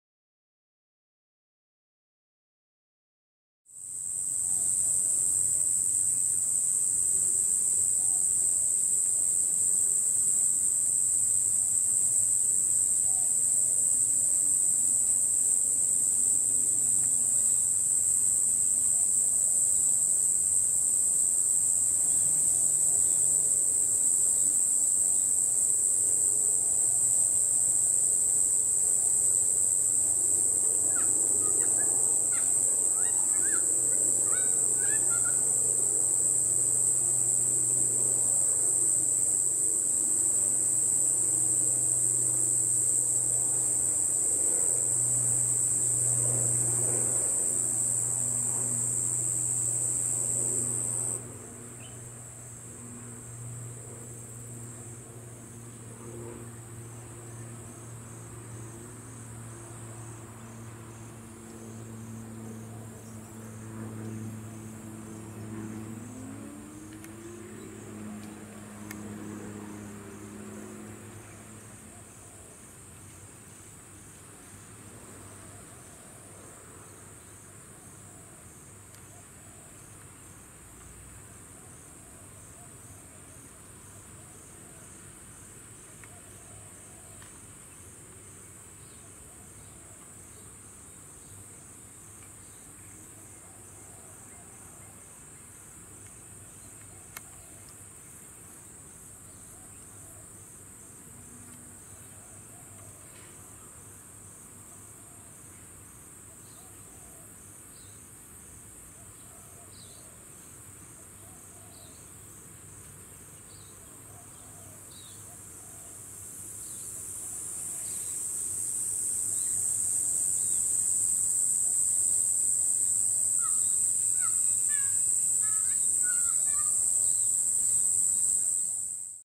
Bar-shouldered Dove & Spotted Dove
Bar-shouldered Dove (Geopelia humeralis) and Spotted Dove, a.k.a. Spotted Turtle-dove (Spilopelia chinensis).  Filmed in a dry artificially landscaped watercourse on the Sunshine Coast, Queensland, in January 2019. This video includes the sound of birdsong and suburban noises